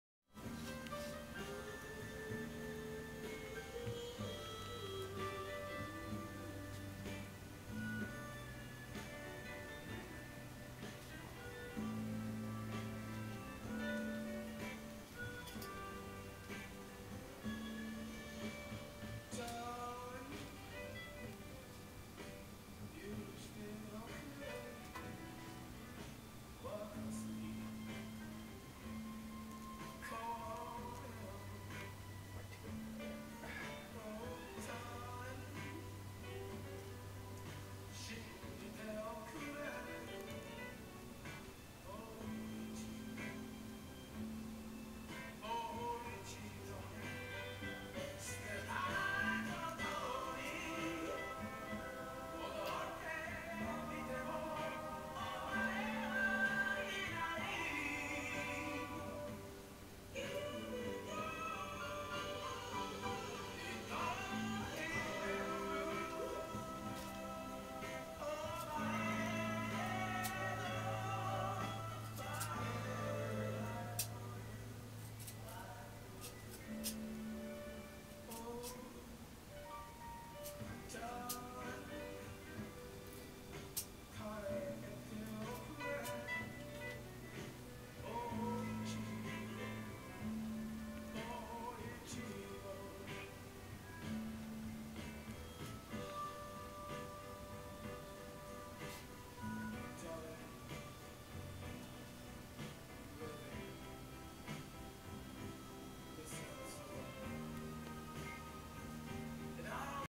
japanese tattoo
japanese tattoo design made by HoriShin Sensie- Nagoya,Japan